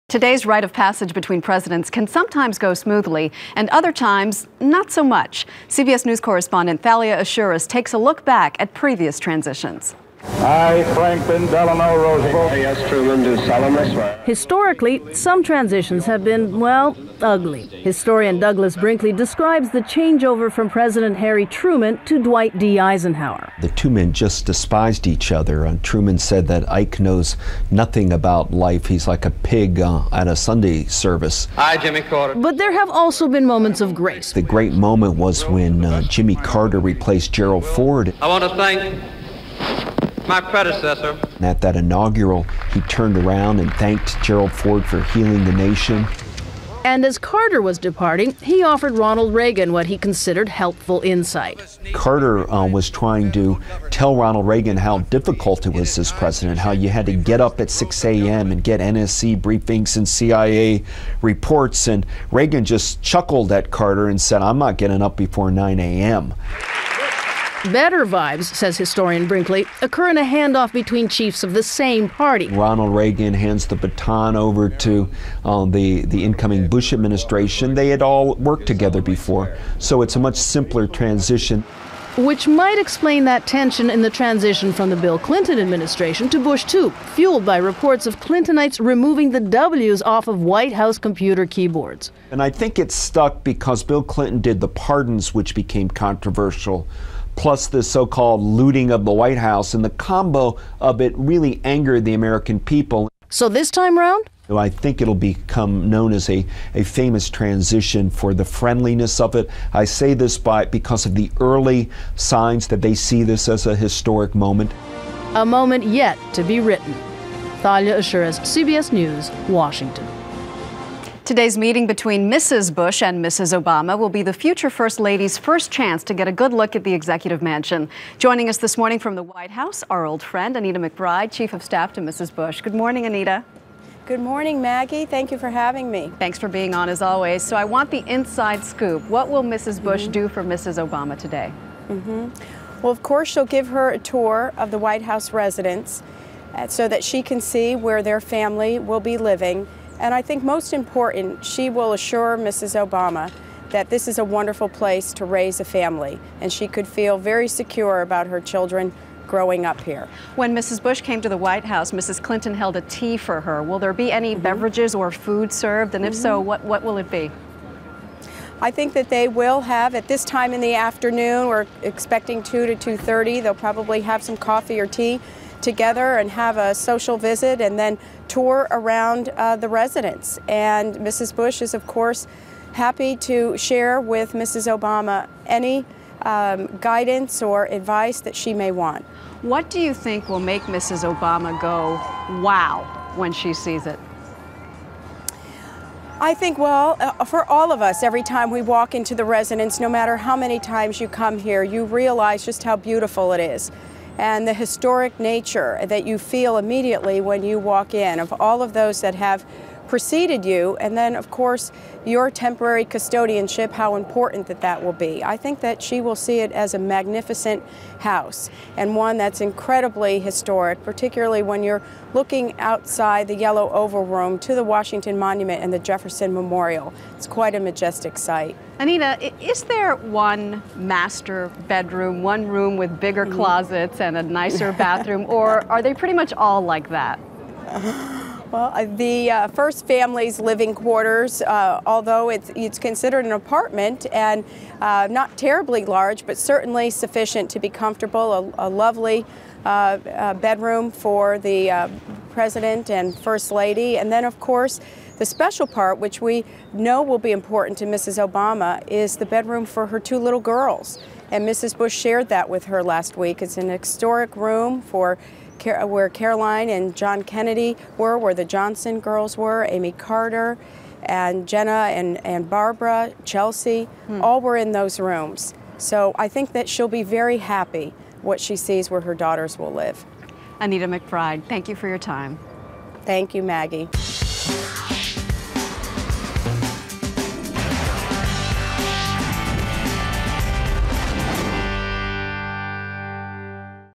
Famous White House Transitions
Passing the presidential baton often included notable moments - both honorable and embarrassing, reports Thalia Assuras. Maggie Rodriguez also talks to Laura Bush's chief of staff about the Obama visit.